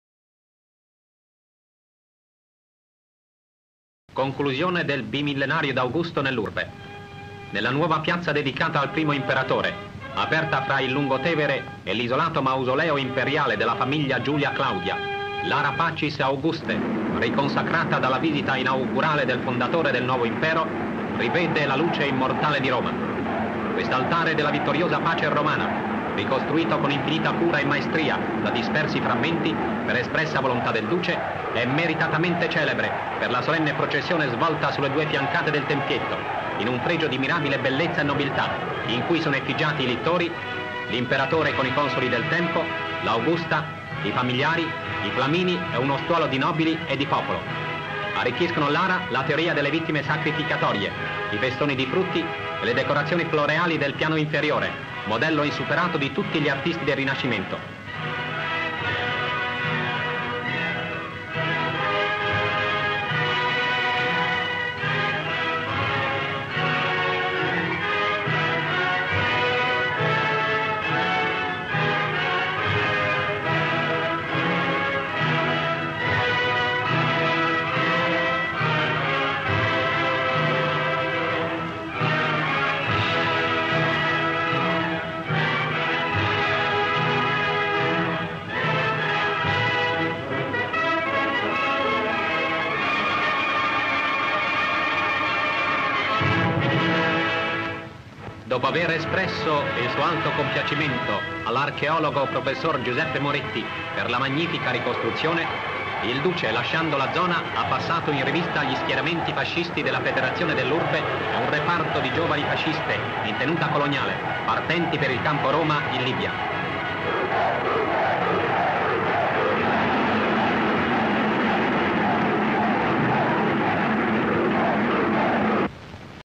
L'inaugurazione dell'Ara Pacis nel nuovo assetto urbanistico.
Descrizione sequenze:reparti di miliziani sono schierati di fronte all'edificio che ospita l'Ara Pacis ; una fila di balilla suona le trombe ; un corteo di automobili arriva ; i vari reparti della milizia applaudono ; i balilla suonano i tamburi ; Mussolini con altre autorità fasciste sale le scale di accesso all'Ara Pacis ; Mussolini e le altre autorità visitano l'interno dell'edificio ; immagine della porta d'ingresso dell'Ara Pacis ; immagini dei bassorilievi intorno all'Ara Pacis ; veduta della ricostruzione dei tratti dei bassorilievi rovinati ; Mussolini con le altre autorità gira intorno all'Ara Pacis ; Mussolini e le altre autorità camminano all'esterno salutato da un reparto di giovani donne fasciste in tenuta coloniale ; immagine dei balilla che suonano i tamburi ; Mussolini e le altre autorità tra la folla esultante ;